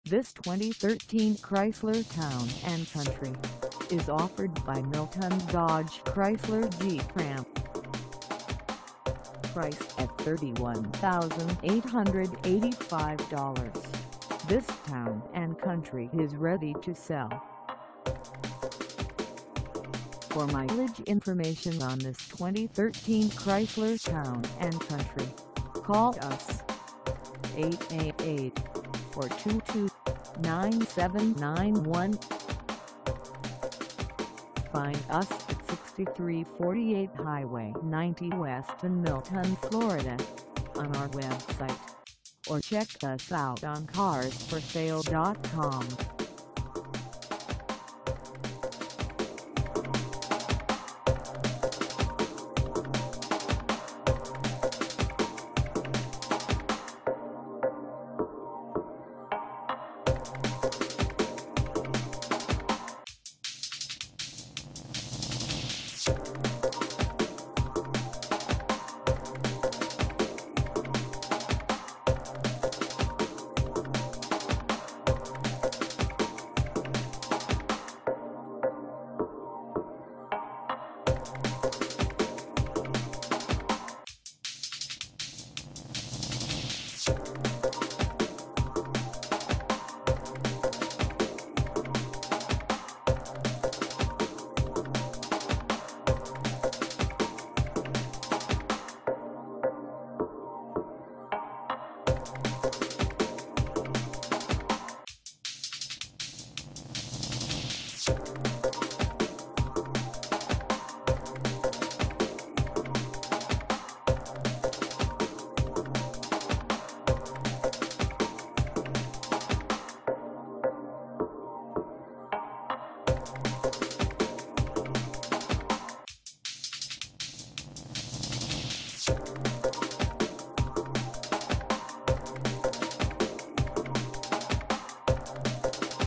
2013 Chrysler Town and Country Touring - for sale in Milton,
MILTON DODGE CHRYSLER JEEP RAM

Come test drive this 2013 Chrysler Town and Country Touring for sale in Milton, FL.